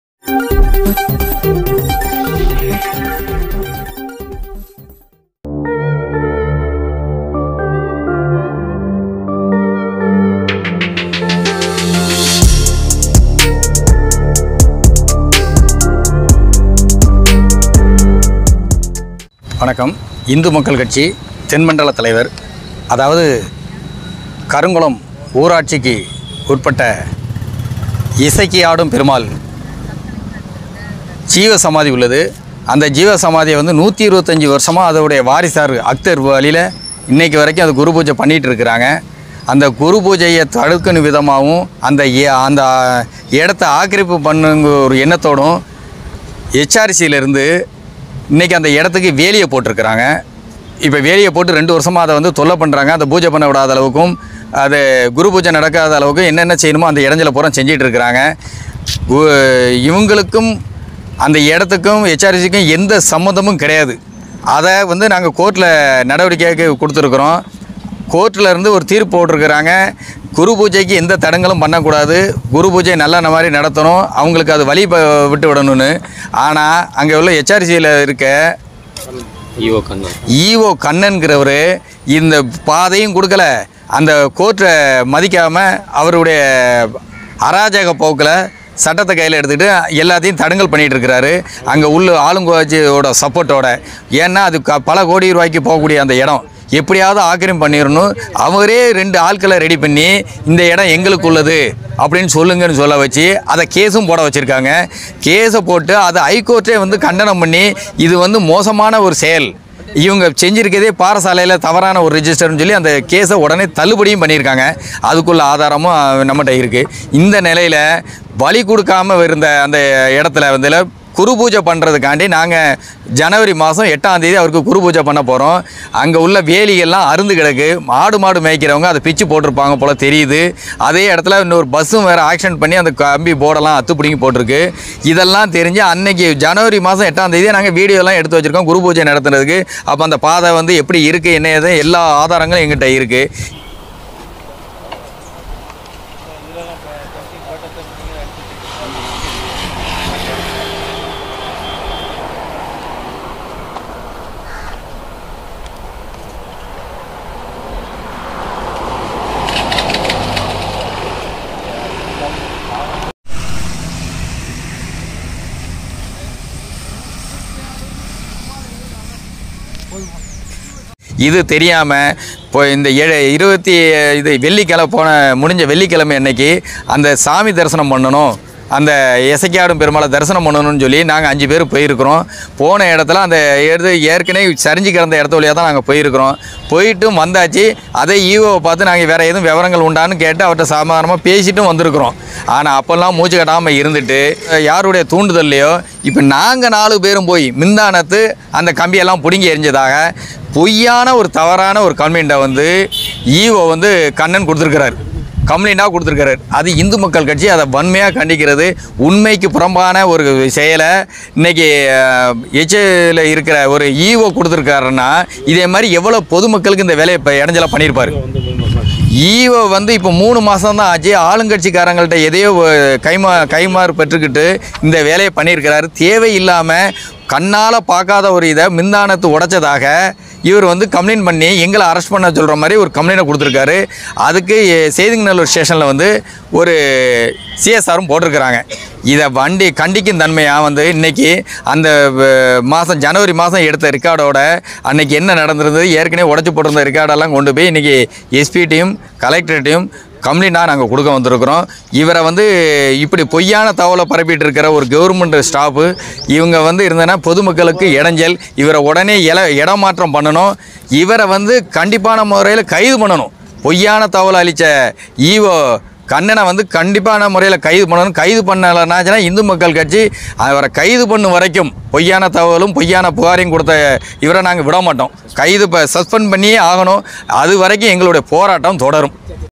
பொய் புகார் அளித்த அறநிலையத்துறை E.O | ஆதாரங்களுடன் உண்மையை உடைத்த இ.ம.க தென்மண்டல தலைவர் | IMK
வீடியோ ஆதாரங்களுடன் உண்மையை உடைத்த இமக தென்மண்டல தலைவர் ராஜா பாண்டியன்.. பொய் புகார் அளித்த அறநிலையத்துறை E.O கண்ணன்..